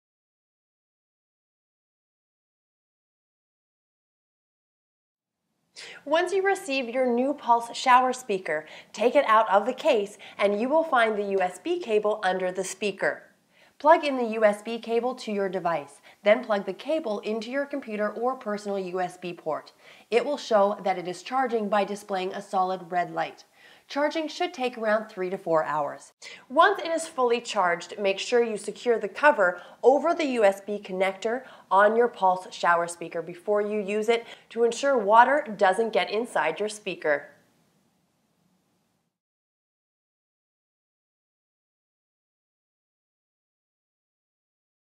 How to charge your Pulse shower speaker!
CSJ Sound Pulse Wireless Waterproof Bluetooth Shower Speaker. Portable Speaker.